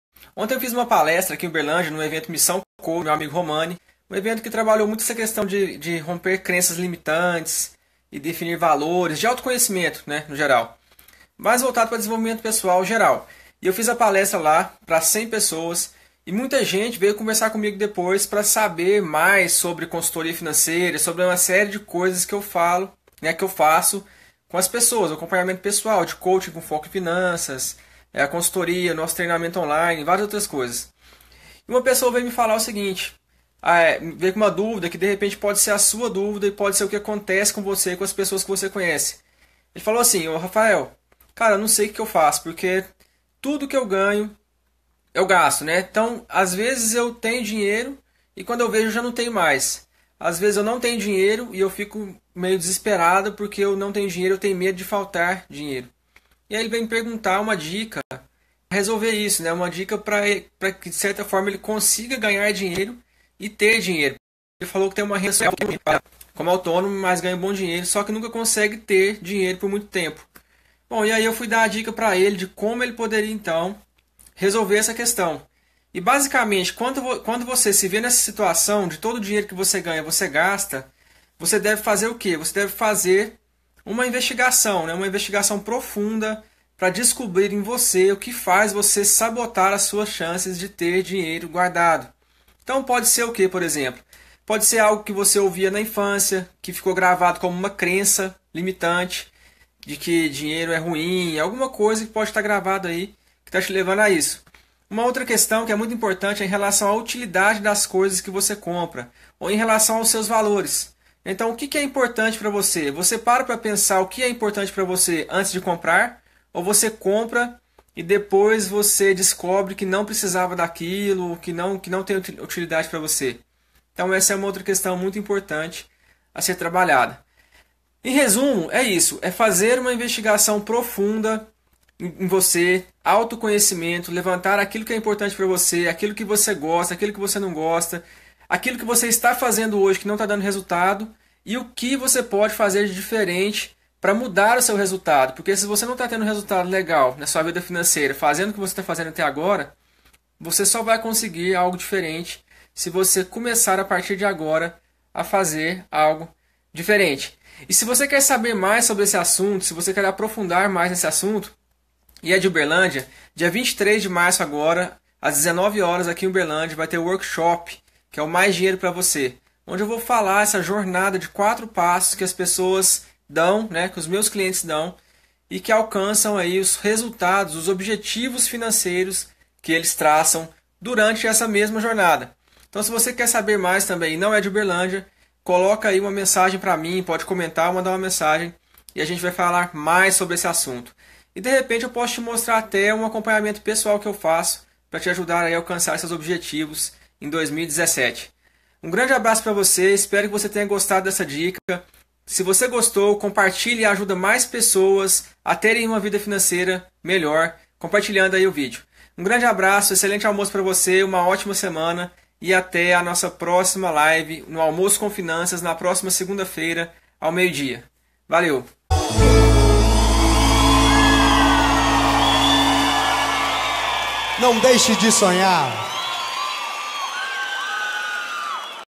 Por que eu gasto todo o dinheiro que eu ganho? Ao Vivo
Neste vídeo eu respondo uma dúvida que recebi no último final de semana, em uma palestra que eu fiz em Uberlândia. Por que eu gasto tudo o que eu ganho?